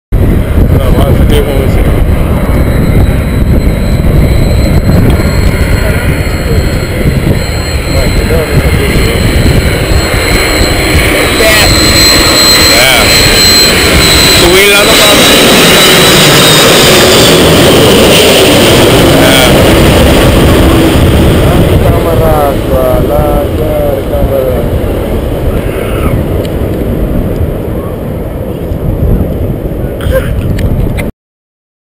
[HD] KLM Airbus 330-200 Landing @ Flamingo Int'l Airport
This was the 1st Flight of the Airbus 330-200 from Amsterdam to Bonaire

"Night Landing"